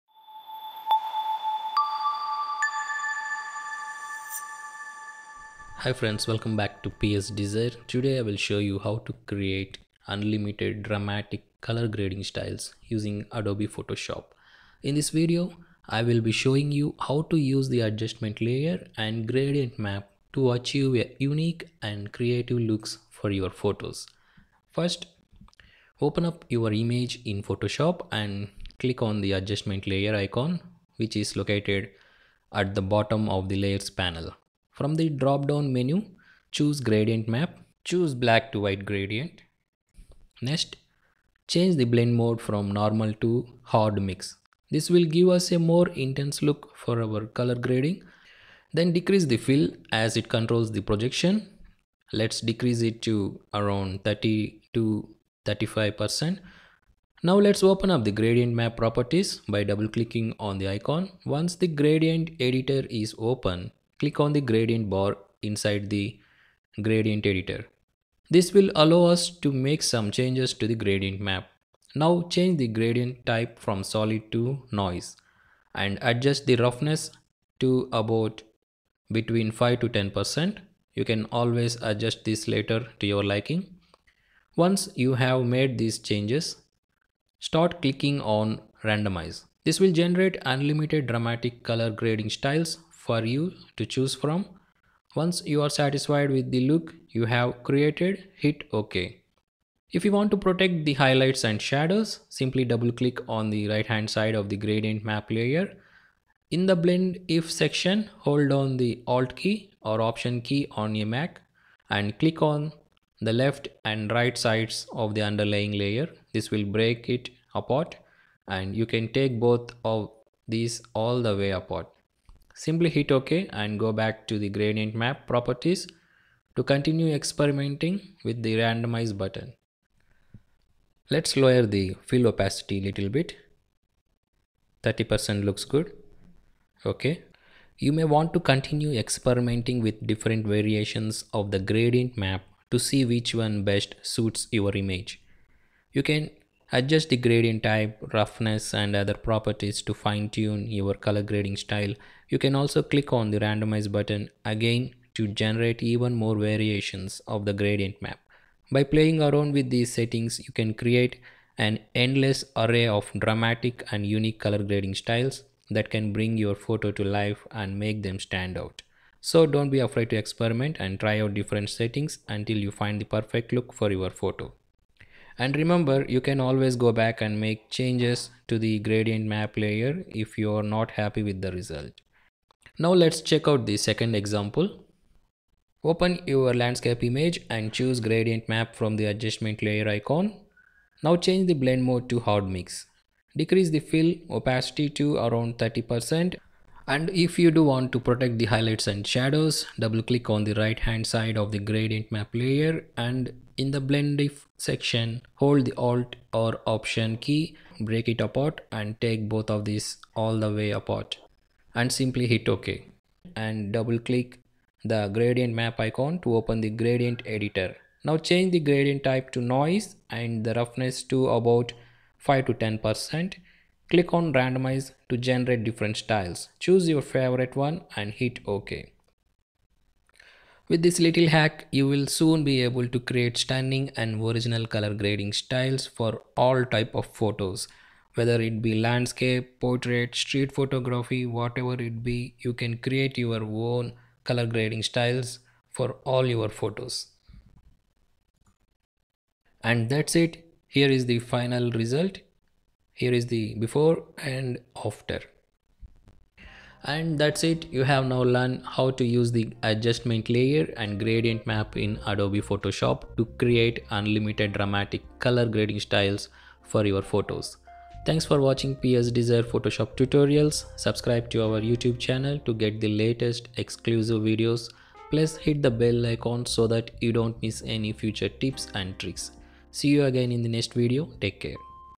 Auto Color Grading - Photoshop Tutorials & Tips
In this photoshop & photography tutorial, I'll show you how to use Adobe Photoshop to create unlimited dramatic color grading styles. First, open your image and choose Gradient Map from the adjustment layer icon. Change the blend mode to Hard Mix, decrease the fill to 32, and double-click the gradient map icon to open the Gradient Editor. Change the gradient type to Noise and the roughness to about 5%. Click on Randomize to generate different styles, choose your favorite, and hit OK. Double-click on the right side of the gradient map layer to protect the highlights and shadows if you want. Hit OK again and go back to the gradient map properties to continue experimenting with the Randomize button. And that's it! Now you can create unique and creative looks for your photos.

And that's it! You've now learned how to use the adjustment layer and gradient map in Adobe Photoshop to create unlimited dramatic color grading styles for your photos.

Photoshop Tutorials for Beginners, Intermediate and Advanced users